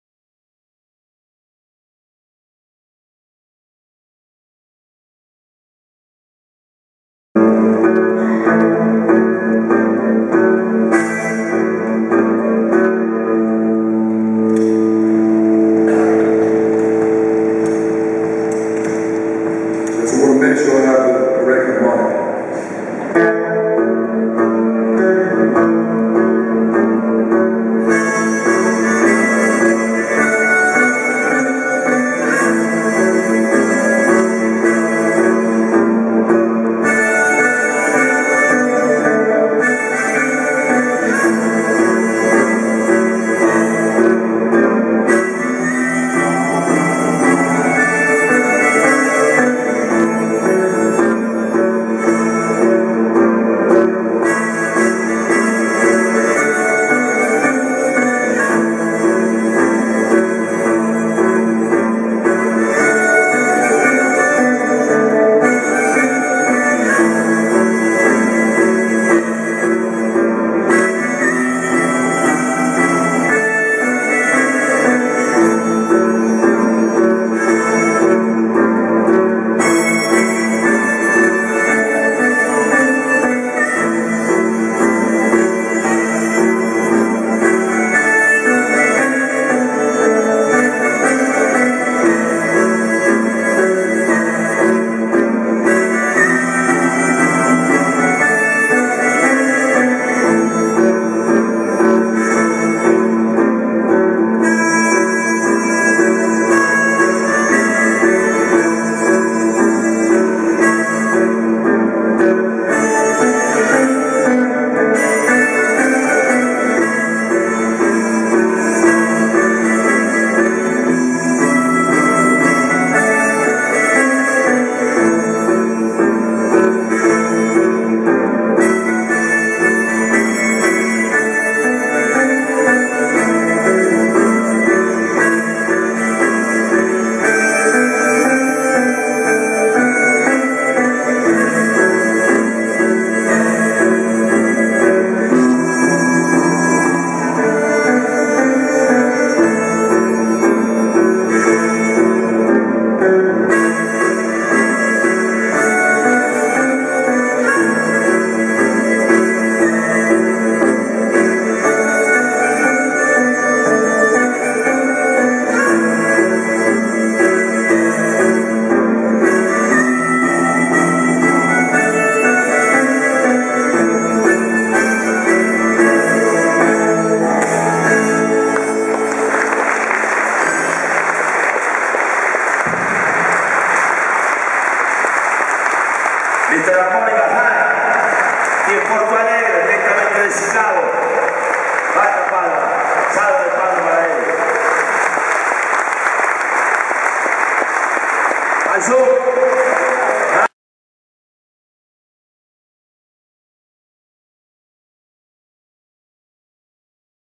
HARMONICA HINDS.SANTANDER CULTURAL.PORTO ALEGRE-RS-BRASIL.02-09-12.By NEUSACALEGARI!
HARMONICA HINDS. (CHICAGO). TRIBUTANDO a BIG WALTER SNAKEY HORTON. ÁTRIO SANTANDER CULTURAL. PORTO ALEGRE-RIO GRANDE DO SUL-BRASIL. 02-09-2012. By NEUSACALEGARI!